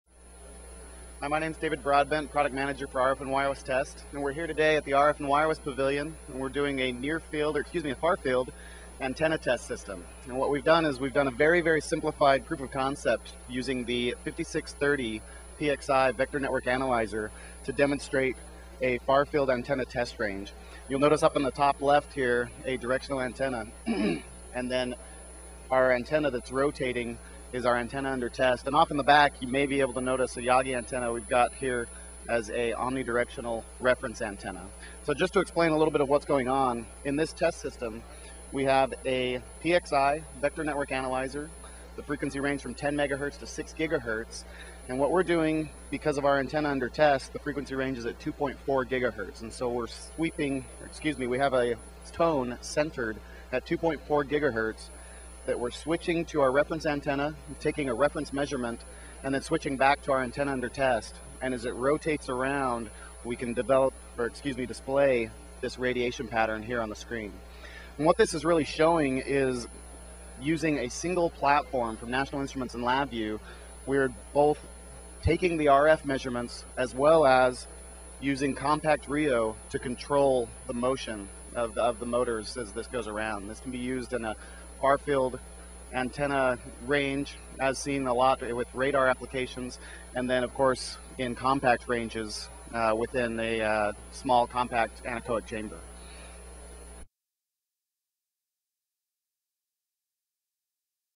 Far Field Antenna Test System Using NI PXI and NI CompactRIO
to learn more about National Instruments RF and Wireless technologies.

This demonstration will show how the NI PXIe-5630 can be combined using NI LabVIEW with NI Compact RIO and NI Motion hardware to create a flexible, fully integrated antenna test system for antenna characterization.